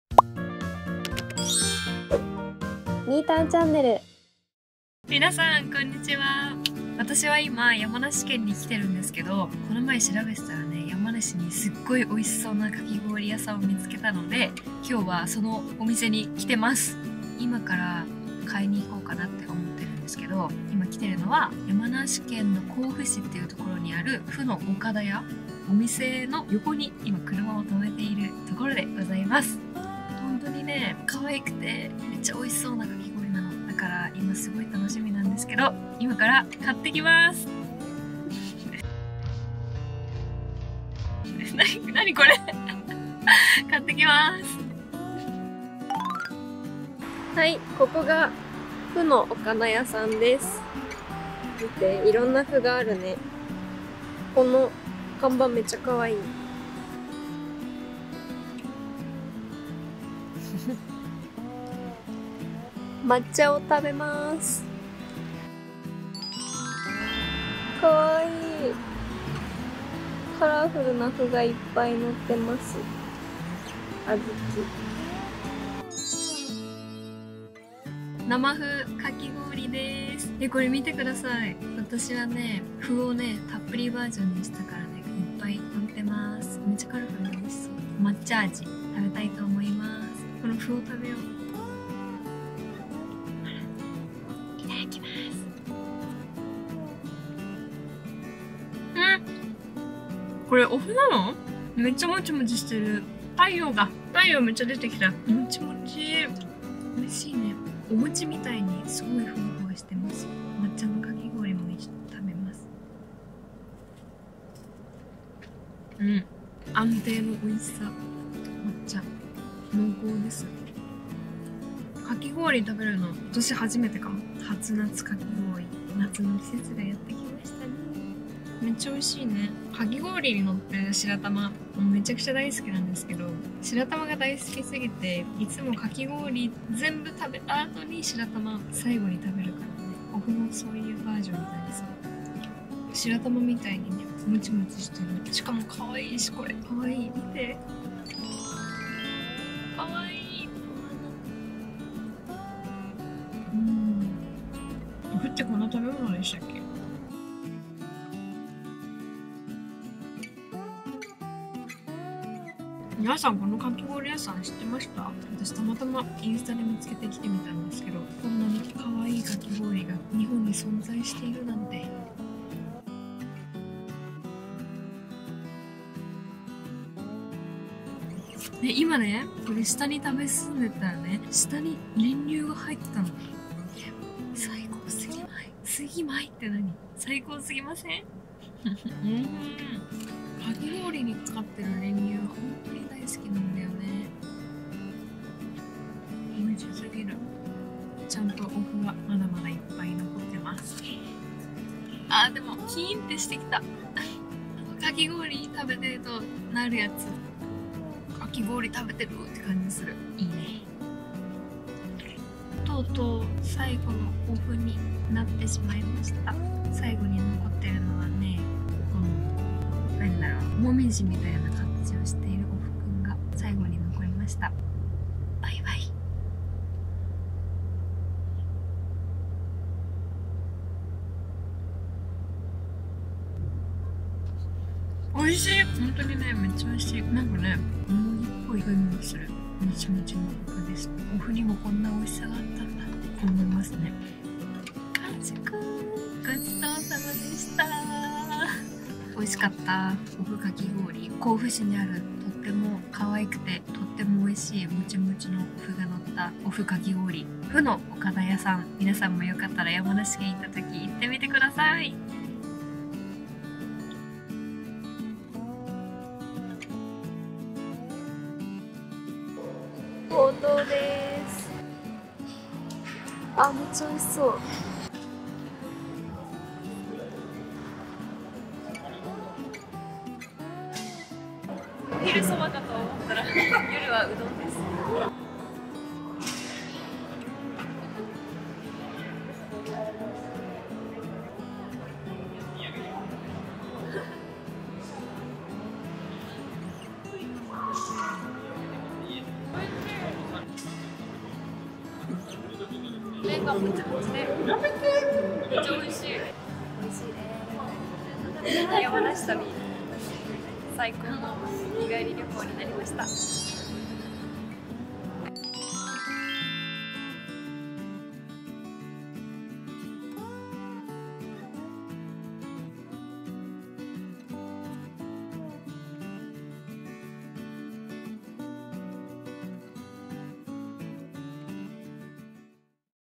最強に可愛いかき氷に出会って幸せ大爆発。【山梨日帰り旅 グルメ編】
【撮影場所】
麩の岡田屋 @山梨県甲府市
ほうとう不動 @山梨県南都留郡

【撮影協力】
友達

山梨日帰り旅後半！

最強にかわいいかき氷に出会えて
わたしは幸せいっぱいです。。。

お麩の可愛さ新食感に加えて
味も美味しいという素晴らしいかき氷。

素敵な出会いがありました🤤❤️

そして旅の締めくくりはほうとう！
やっぱり山梨に来たらほうとうは外せないよね！
このお店は前にも行ったことがあって大好きなところです。

みんなもこの動画を見て
少しでも旅気分を味わってもらえたら嬉しいなぁ

とにかくグルメ旅な一日でした🌞

今回も観てくれてありがとう！

Twitter、Instagramはこちらから⤵︎⤵︎